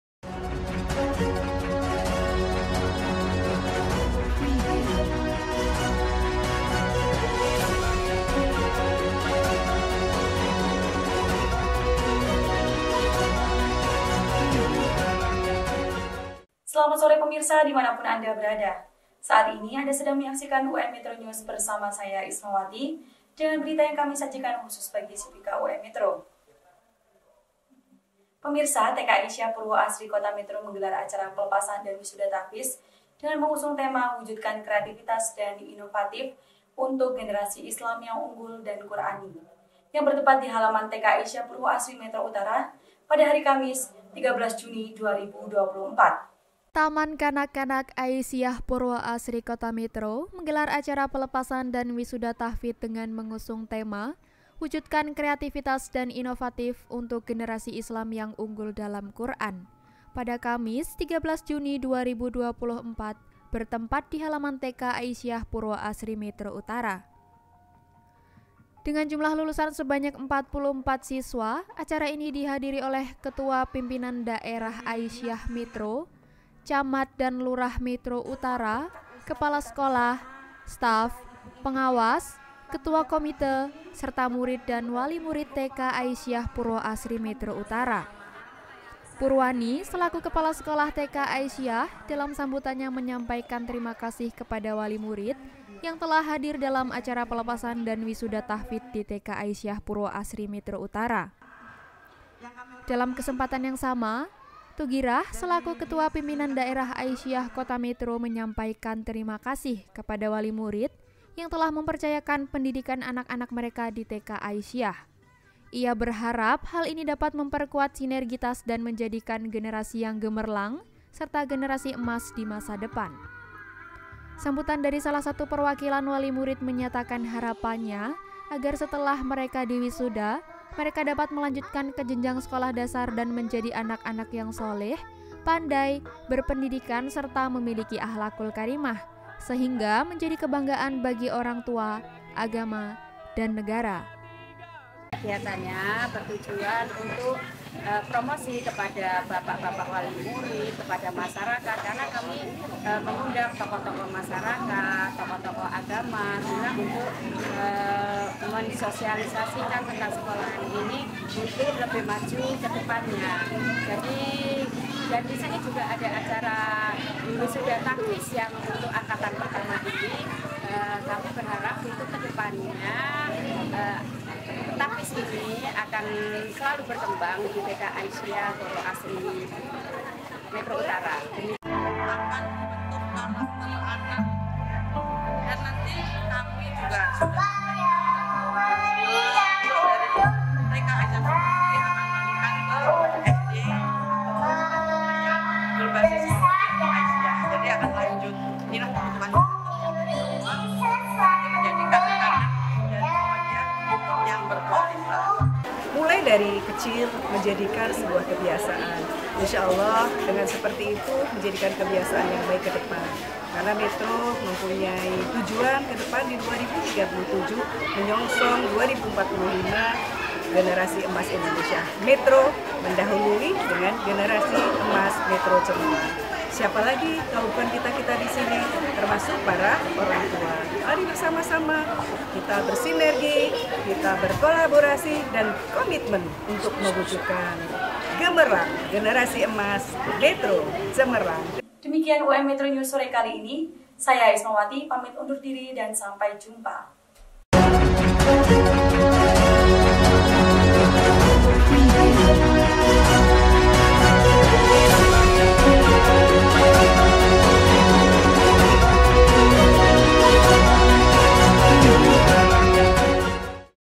[NEWS] WISUDA TAHFIDZ TK AISYIYAH PURWOASRI METRO UTARA, MENUJU MASA DEPAN YANG GEMERLANG.
Wisuda Tahfidz TK Aisyiyah Purwoasri Metro Utara Pada Kamis, 13 Juni 2024

Pengawas: M. Nur, M.Kom.I
Produser: Bungsudi, M.Pd

1. Reporter: Rindy Citra Andini
2. Asisten Reporter: Nuki Puspita Sari, Zulfa Sajida Khoir
3. Editor: Risma Nella Zannati
4. Cameraman: M. Ibnu Ardi Septian
5. Presenter : Ismawati

Find Us On:
Tik-Tok : @um_metro